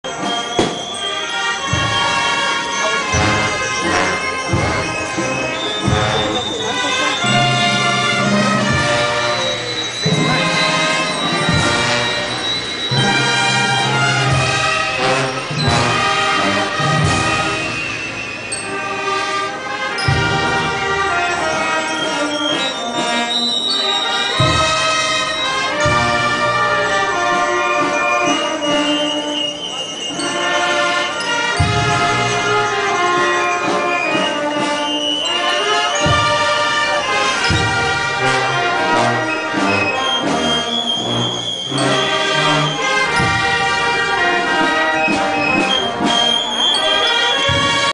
Primer Domingo de Cuaresma - Procesion de Jesus Nazareno de la Salvacion, Santa Catarina Bobadilla
The first procession passing through Santa Ana/Antigua on February 21, 2010.

This procession lasted 11-12 hours, starting from Santa Catarina Bobadilla at 11 am and was scheduled to return by 10 pm although they only left Antigua at 8.45 pm!

Apologies for the grainy nature of the video, the camera has only recently returned from major surgery and looks like it needs a bit more to get it up to scratch!

If you enjoyed this video there are more and we will be uploading frequently through Semana Santa.